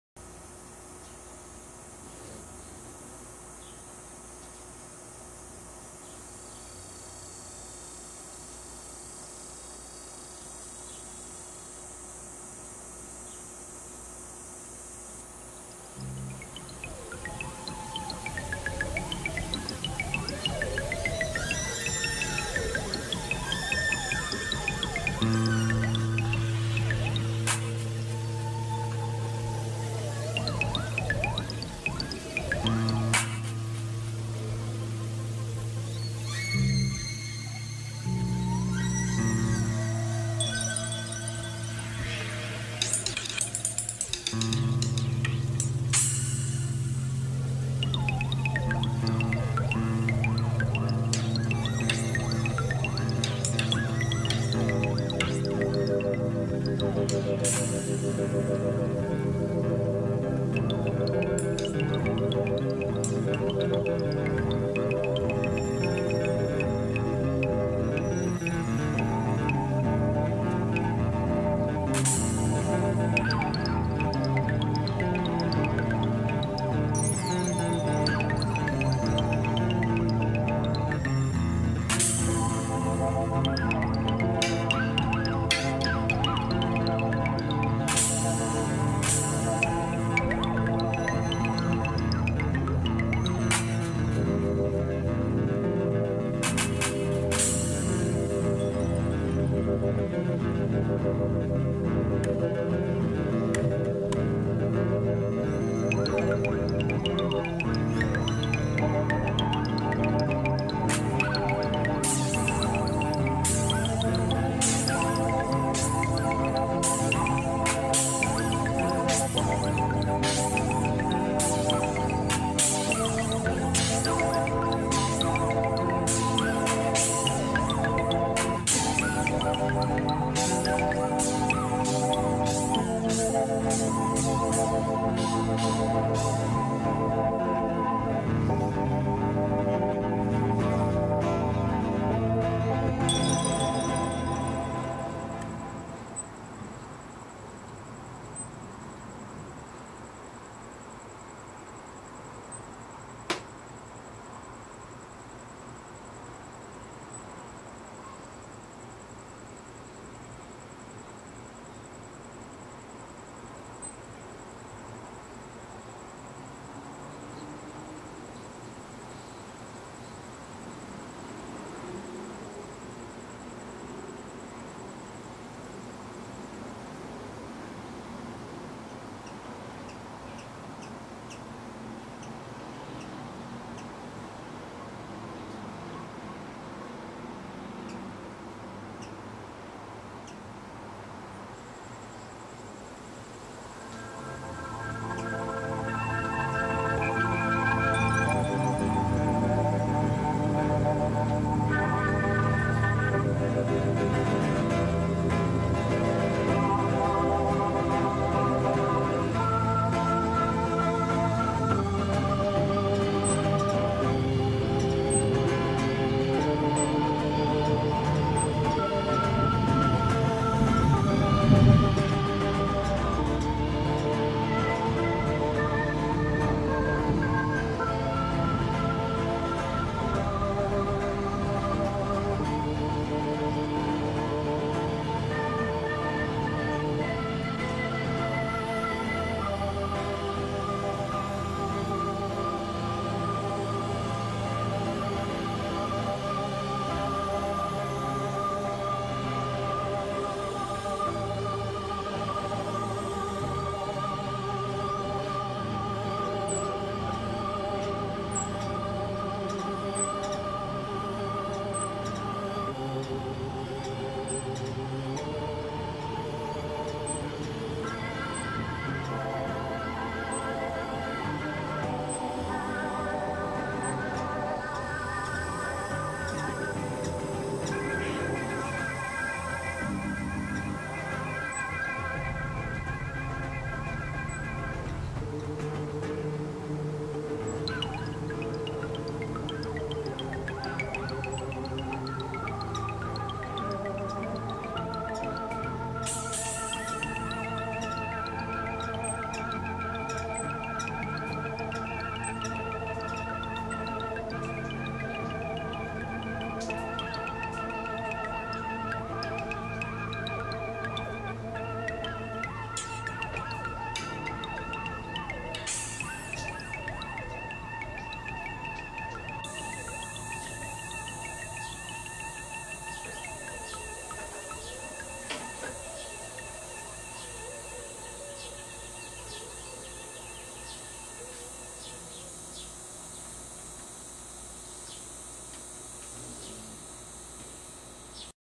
Hummingbird Nest Summer 2010
Momma hummingbird feeds and cares for her young.